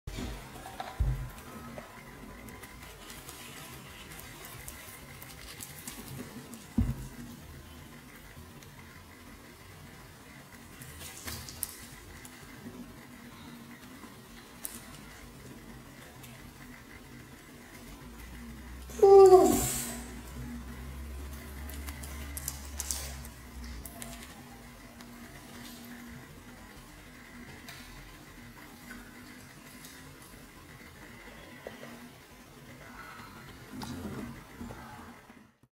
Sony A6500 SLOW MOTION
Slow Motion with the Sony A6500 in 50% with FCPX.

The lens that I used was the Sigma 16mm 1.4.

Recorded in 100 fps and mount on FCPX in 25 fps.

Bread made with Thermomix